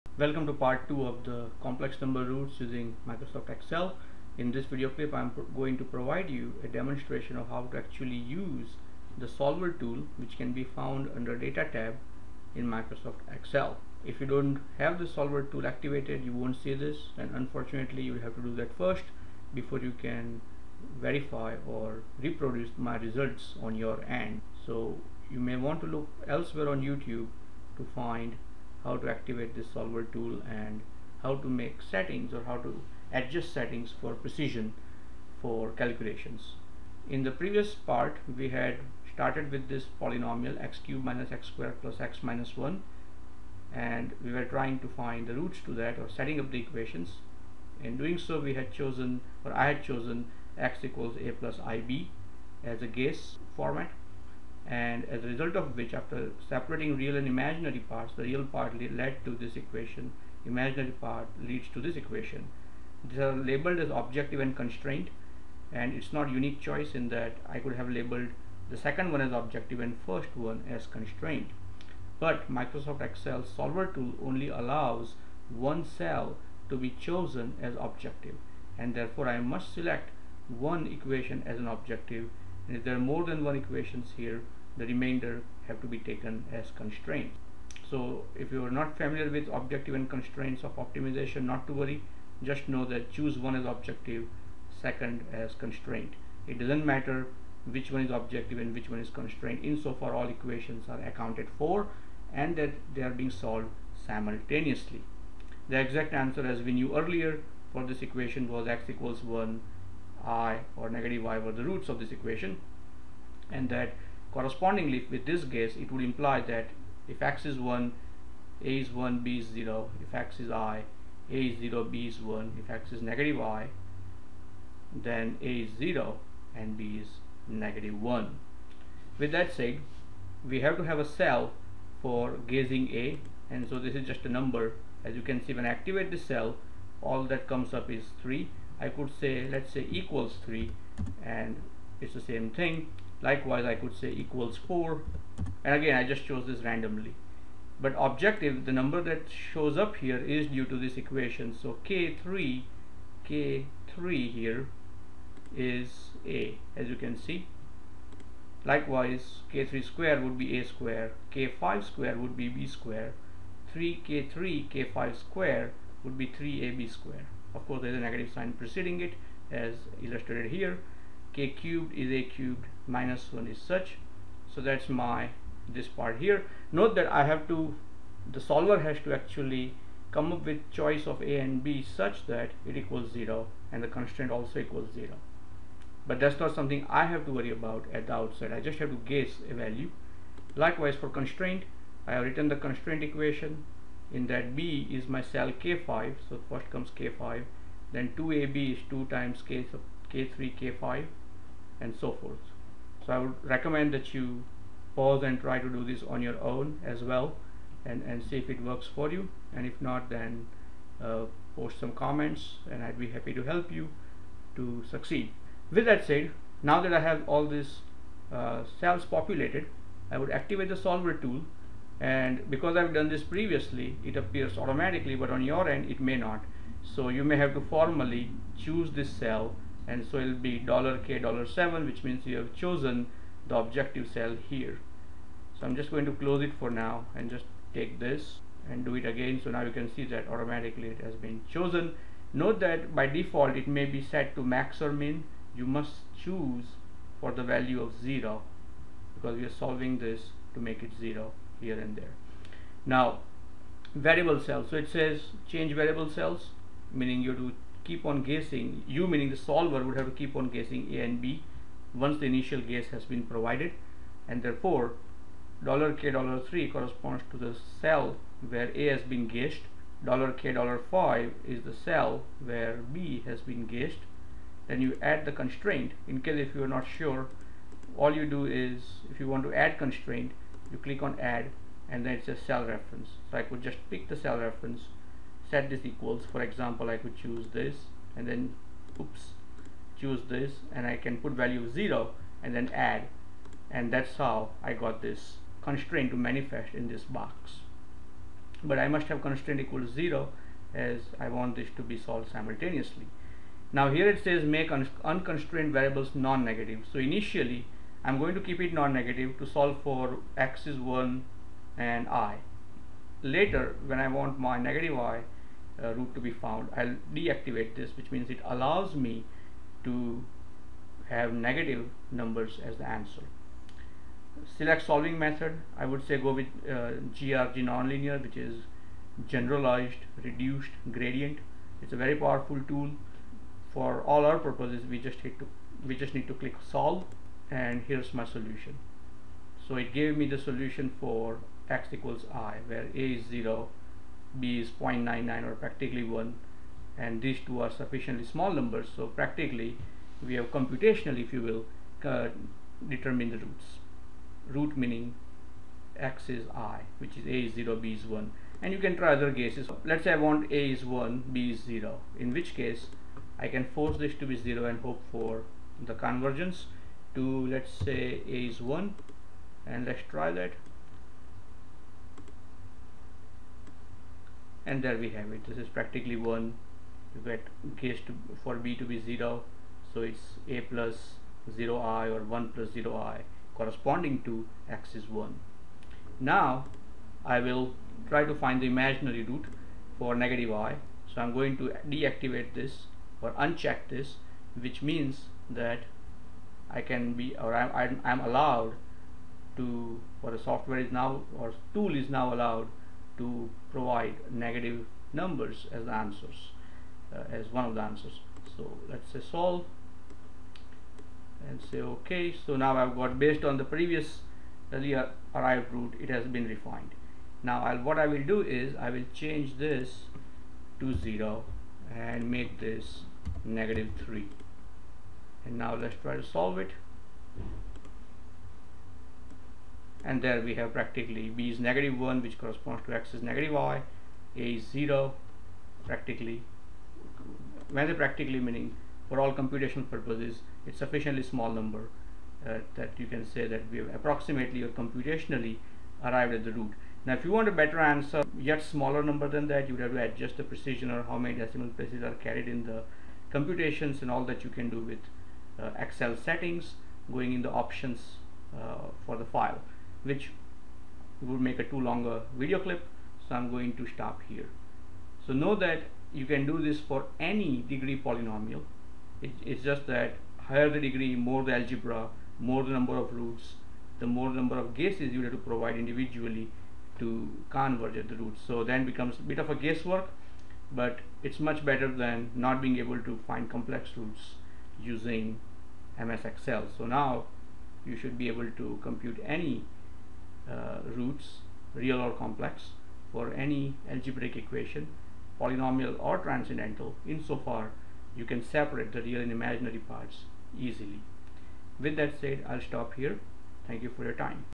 COMPLEX ROOTS USING MS EXCEL PART 2
This video describes a method for computation of complex roots using Solver tool in MS Excel. Although MS Excel has IMSUM, IMPOWER, etc., it becomes rather tricky to employ these for rootfinding. This is where the method presented (although tedious) helps.